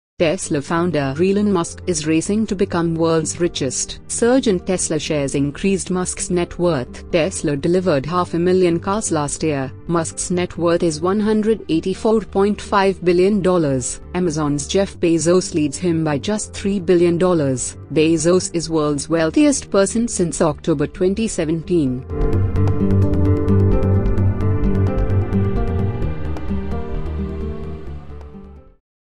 Tesla founder Elon Musk is racing to become world's richest
Surge in Tesla shares increased Musk's net worth
Tesla delivered half-a-million cars last year
Musk's net worth is $184.5 billion
Amazon's Jeff Bezos leads him by just $3 billion
Bezos is world's wealthiest person since Oct. 2017